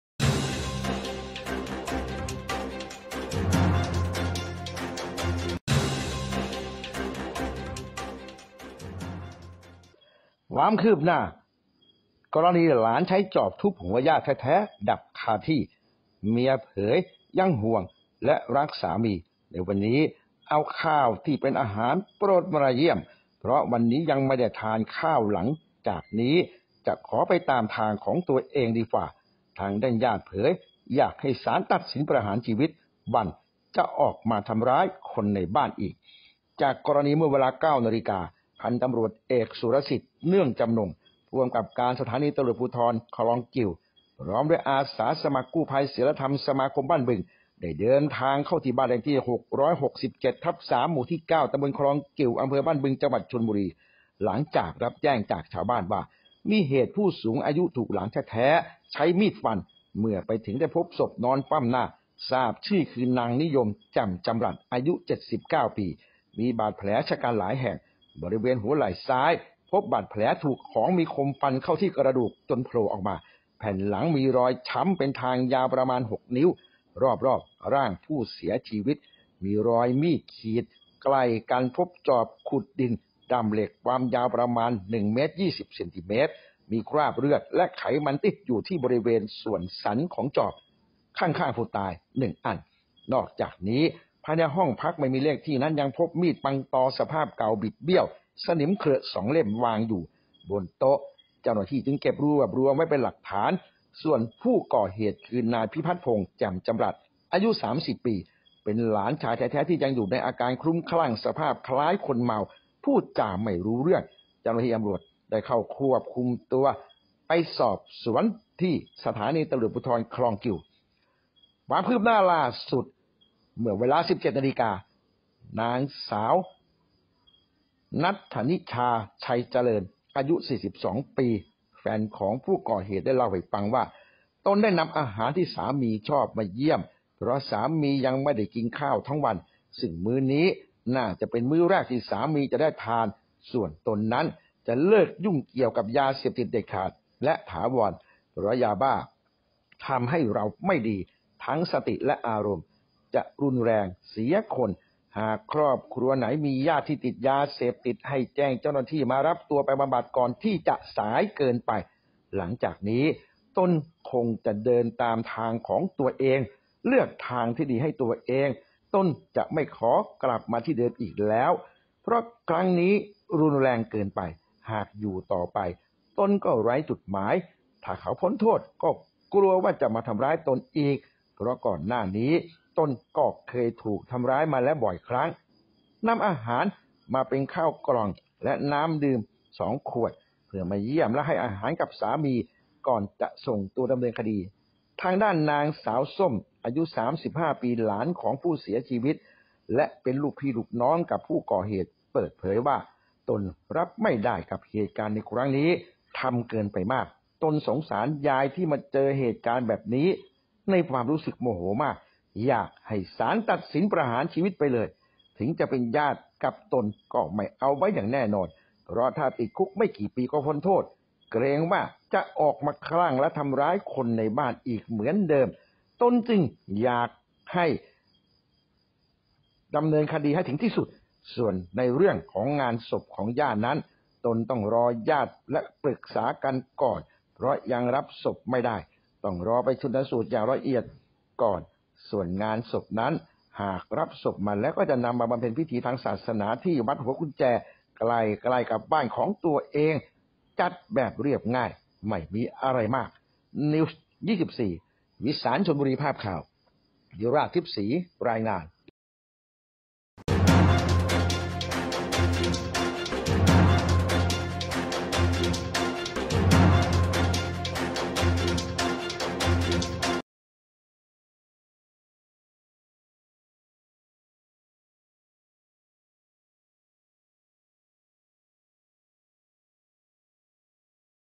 ชลบุรี คืบหน้าหลานใช้จอบทุบหัวย่าแท้ๆดับคาที่ เมียเผยยังห่วงและรักสามีวันนี้เอาข้าวที่เป็นอาหาร
ผู้ประกาศ……ที่ชลบุรี   คืบหน้าหลานใช้จอบทุบหัวย่าแท้ๆดับคาที่ เมียเผยยังห่วงและรักสามีวันนี้เอาข้าวที่เป็นอาหารโปรดมาเยี่ยมเพราะวันนี้ยังไม่ได้ทานข้าวหลังจากนี้จะขอไปตามทางตัวเองดีกว่า ทางด้านญาติเผยอยากให้ศาลตัดสินประหารชีวิตหวั่นจะออกมาทำร้ายคนในบ้านอีก
เทปข่าว…….จากกรณีเมื่อเวลา 09.00 น.พ.ต.อ.สุรสิทธิ์  เนื่องจำนงค์ ผู้กำกับการ สภ.คลองกิ่ว พร้อมด้วยอาสากู้ภัยศีลธรรมสมาคมบ้านบึง เดินทางเข้าที่บ้านเลขที่ 667/3 หมู่ที่9 ตำบลคลองกิ่ว อำเภอบ้านบึง จังหวัดชลบุรี  หลังรับแจ้งจากชาวบ้านว่า มีเหตุผู้สูงอายุถูกหลานแท้ๆใช้มีดฟัน เมื่อไปถึงพบศพนอนคว่ำหน้า ทราบชื่อ คือ นางนิยม  แจ่มจำรัส อายุ 79 ปี มีแผลฉกรรจ์หลายแห่ง  บริเวณหัวไหล่ซ้ายพบบาดแผลถูกของมีคมฟันเข้ากระดูกโผล่  แผ่นหลังมีรอยช้ำเป็นทางยาวประมาณ  6 นิ้ว  รอบๆร่างผู้เสียชีวิตมีรอยมีดขีด ใกล้กันพบจอบขุดดินด้ามเหล็กความยาวประมาณ 1.20 เมตร มีคราบเลือด และไขมันติดอยู่บริเวณส่วนสันของจอบ อยู่ข้างๆผู้ตาย 1 อัน  และนอกจากนี้ภายในห้องพักไม่มีเลขที่ยังพบมีดปังตอสภาพเก่าบิดเบี้ยว สนิมเขรอะ  2 เล่ม วางอยู่บนโต๊ะ เจ้าหน้าที่จึงเก็บรวบรวมไว้เป็นหลักฐาน ส่วนผู้ก่อเหตุคือนายพิพัฒน์พงศ์  แจ่มจำรัส  อายุ 30 ปี เป็นหลาน ที่ยังอยู่ในอาการคลั่ง สภาพคล้ายคนเมา พูดจาไม่รู้เรื่อง เจ้าหน้าที่ตำรวจได้เข้าควบคุม ทำไปสอบสวนต่อที่ สภ.คลองกิ่ว
    คืบหน้าล่าสุดวันนี้ 20 ตุลาคม 2565 เวลา17.00น.ส่วนนางสาวนางสาวณัฏฐณิชา ไชยเจริญ อายุ 42 ปี (เบลอหน้า ด้วยครับ) แฟนของผู้ก่อเหตุได้เล่าว่าวันนี้ตนนำอาหารที่สามีชอบมาเยี่ยมเพราะสามียังไม่ได้กินข้าวทั้งวันซึ่งมื้อนี้เป็นมื้อแรกที่สามีจะได้ทานส่วนตนนั้นจะเลิกยุ่งเกี่ยวกับยาเสพติดเด็ดขาดและถาวรเพราะยาบ้าทำให้เราไม่ดีทั้งสติและอารมณ์จะรุนแรงเสียคนหากครอบครัวใหนมีญาติที่ติดยาเสพติดให้แจ้งเจ้าหน้าที่มารับตัวไปบำบัดก่อนจะสายเกินไปหลังจากนี้ตนคงจะเดินตามทางของตัวเองเลือกทางที่ดีให้ตัวเองตนจะไม่ขอกลับมาที่เดิมอีกแล้วเพราะครั้งนี้รุนแรงเกินไปหากอยู่ต่อตนก็ไร้จุดหมายถ้าเขาพ้นโทษก็กลัวว่าจะมาทำร้ายตนอีกเพราะก่อนหน้านี้ตนก็เคยถูกทไร้ายมาบ่อยนำอาหารเป็นข้าวกล่องและน้ำดื่มจำนวน2ขวดเพื่อมาเยี่ยมและให้อาหารกับสามีก่อนจะส่งตัวดำเนินคดีทางด้านนางสาวส้ม อายุ 35 ปี หลานของผู้เสียชีวิตและเป็นลุกพี่ลูกน้องกับผู้ก่อเหตุได้เปิดเผยว่าตนรับไม่ได้กับเหตุการณ์ในครั้งนี้ทำเกินไปมากตนสงสารยายที่มาเจอเหตุการณ์แบบนี้ในความรู้สึกโมโหมากอยากให้ศาลตัดสินประหารชีวิตไปเลยถึงจะเป็นญาติกันตนก็ไม่เอาไว้แน่นอนเพราะถ้าติดคุกไม่กี่ปีก็พ้นโทษกลัวว่าจะออกมาคลั่งทำร้ายคนในบ้านอีกเหมือนเดิมตนจึงอย่ก ส่วนเรื่องงานศพของย่านั้นตนต้องรอญาติปรึกษากันก่อนเพราะยังรับศพไม่ได้ต้องรอไปชันสูตรอย่างละเอียดก่อนส่วนงานศพนั้นหากรับศพมาแล้วจะนำมาบำเพ็ญพิธีทางศาสนาที่วัดหัวกุญแจ ใกล้ๆบ้านตัวเองและจัดแบบเรียบง่ายไม่มีอะไรเยอะ                                          วิศาล///ชลบุรีปู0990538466